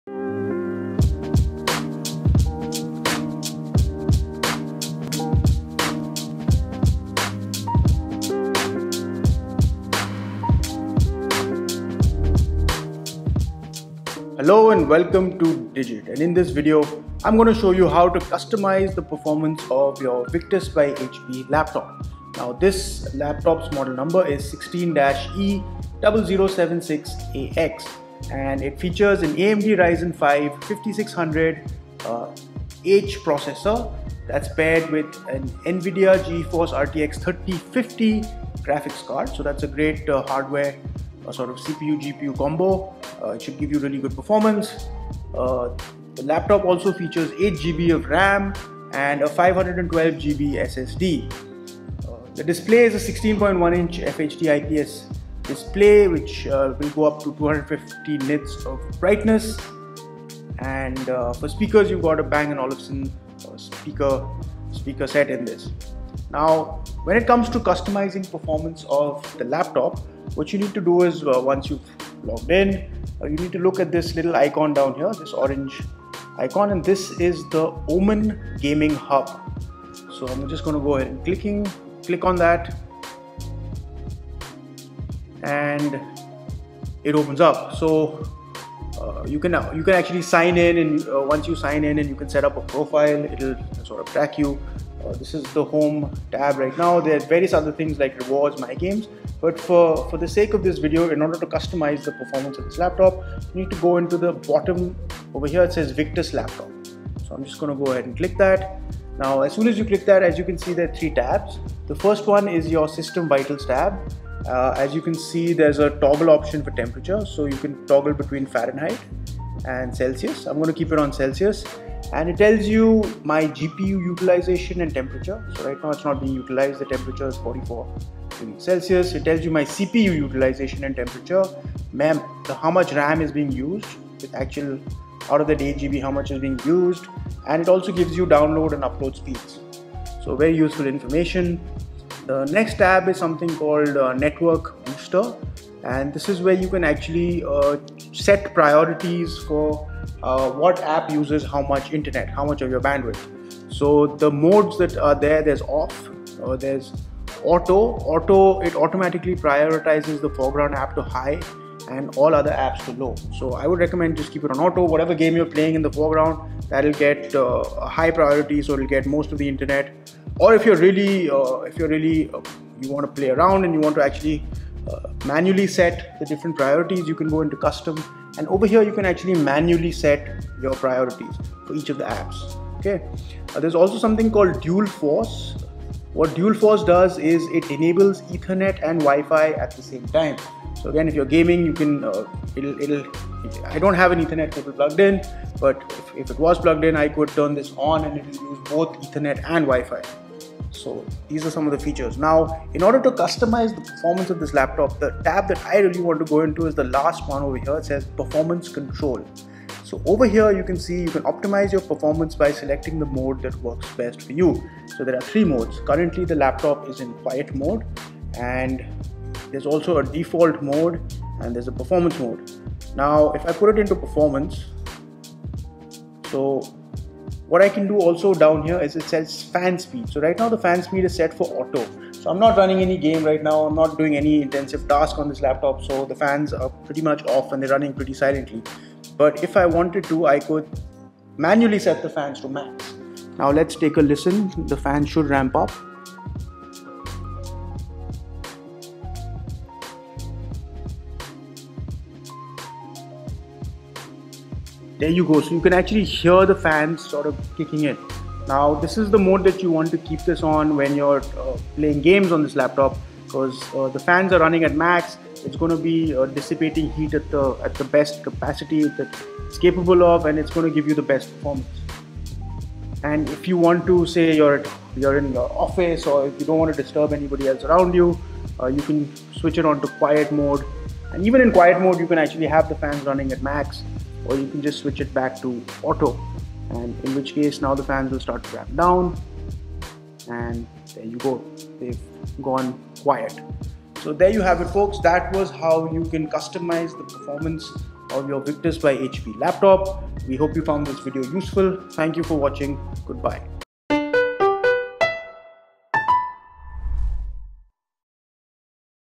How to Customize Performance of the Victus by HP Gaming Laptop 16-e0076AX
About the Victus by HP Gaming Laptop 16-e0076AX:
Processor: 5th Gen AMD Ryzen 5 5600H (3.3GHz base clock speed, up to 4.2 GHz Max Boost Clock, 16MB L3 cache, 6 core, 12 threads)
Memory & Storage: 8 GB DDR4-3200 MHz RAM, Up to 32 GB DDR4-3200 SDRAM (2 x 16 GB) | Storage: 512 GB PCIe NVMe TLC M.2 SSD
Display: 40.9 cm (16.1") diagonal, FHD (1920 x 1080), 60 Hz Refresh Rate, IPS, edge-to-edge, micro-edge, 250 nits, 137 ppi, Color Gamut: 45% NTSC
Graphics & Networking: NVIDIA GeForce RTX 3050 (4 GB GDDR6 dedicated) | Realtek Wi-Fi 6 (2x2) and Bluetooth 5.2 combo (Supporting Gigabit data rate), MU-MIMO supported, Miracast Compatible
Operating System & Software: Pre-loaded Windows 10 Home with lifetime validity,Free upgrade to Windows 11* when available(Refer Microsoft Website) | Pre-installed Microsoft Office Home & Student 2019 | Alexa Built-in

How to Customize Performance of the Victus by HP Gaming Laptop 16-e0076AX,Victus by HP Gaming Laptop 16-e0076AX,hp victus 16,hp victus,hp